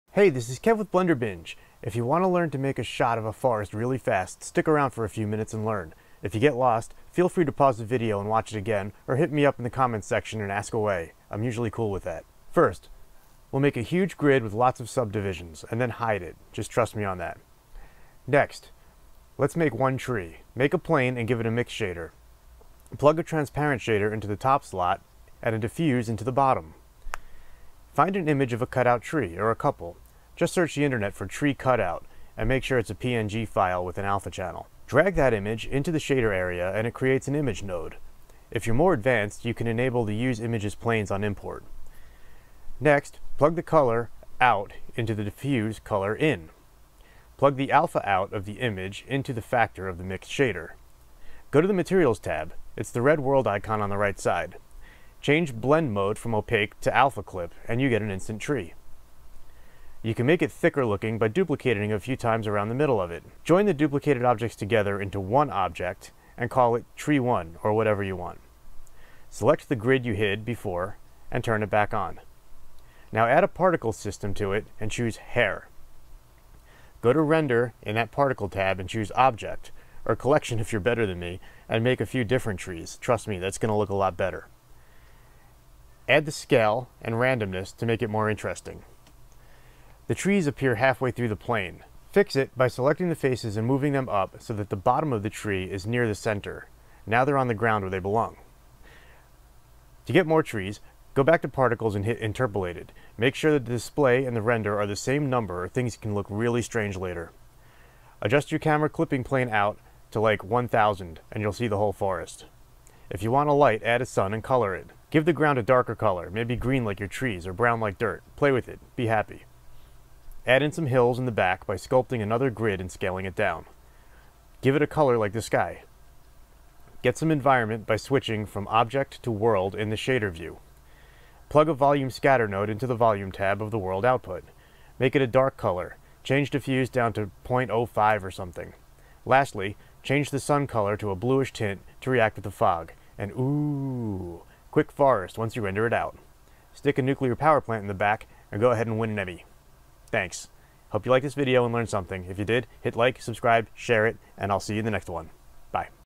Large Scale Forest Flyover in Blender 2.8 - Quick and Dirty
This is a really fast tutorial of creating a fake drone shot over a forest in Blender 2.8.

Due to the speed of this tutorial, please feel free to hit me up on the comments if you have questions.  Remember, this is quick and dirty, and meant to be a starting point!  Don't worry, I'll still be making longer tutorials too lol!

- 📲👍Grab a free CG Animation Time Saver eBook here, learn about the industry and get lots of shortcuts